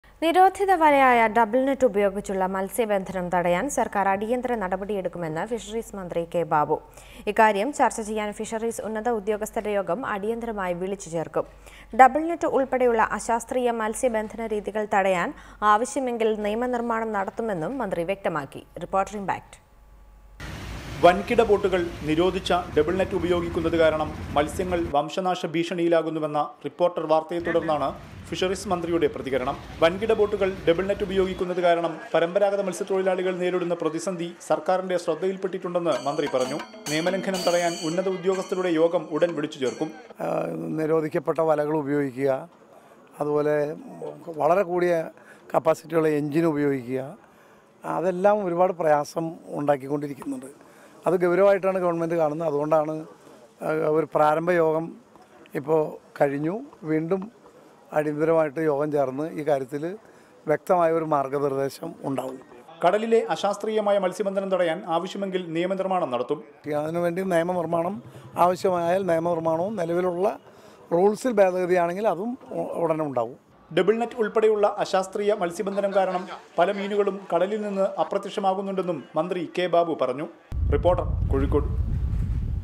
K Babu on Fisheries - Reporter Impact
നിരോധിത വലയായ ഡബിള്‍നെറ്റ് ഉപയോഗിച്ചുള്ള മത്സ്യബന്ധനം തടയാന്‍ സര്‍ക്കാര്‍ അടിയന്തിര നടപടിയെടുക്കുമെന്ന് ഫിഷറീസ് മന്ത്രി കെ.ബാബു.